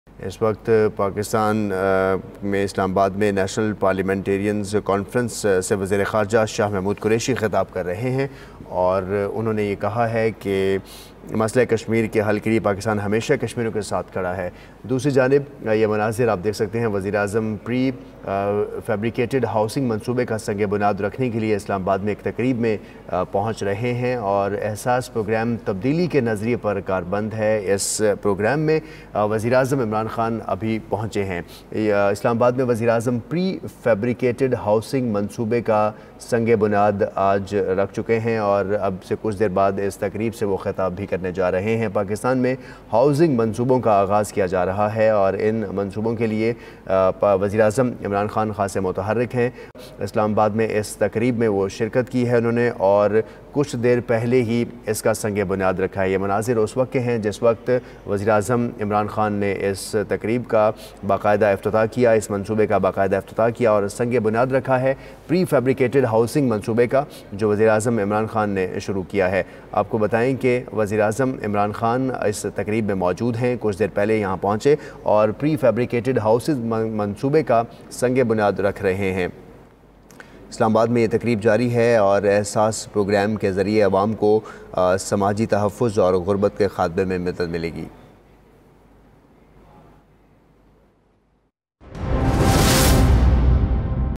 PM Imran Khan inaugurates Prefabricated Housing Project in Pakistan | 19 September 2019
PM Imran Khan Inaugurates pre-fabricated housing Project in Pakistan  | 18 September 2019 |